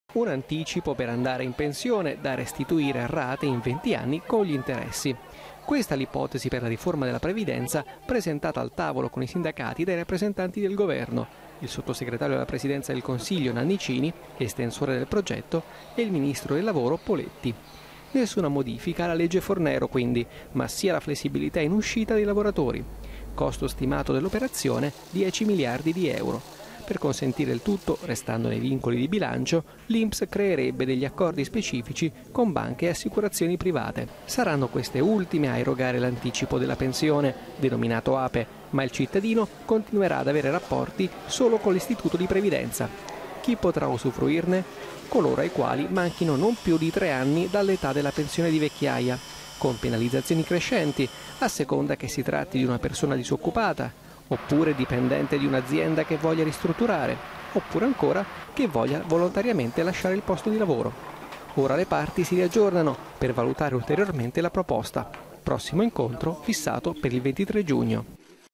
Uscita anticipata dal mondo del lavoro: le ultime novità dopo l'incontro tra Governo e sindacati
Nuovo round di incontri tra governo e sindacati per discutere dell'uscita anticipata dal mondo del lavoro. L'ipotesi dell'esecutivo per chi va prima in pensione e' quella di un prestito che dovra' essere restituito con rate fino a 20 anni con gli interessi. Intanto viene ventilata l'ipotesi di pensione anticipata anche per i dipendenti pubblici. Servizio di Pierluigi Vito.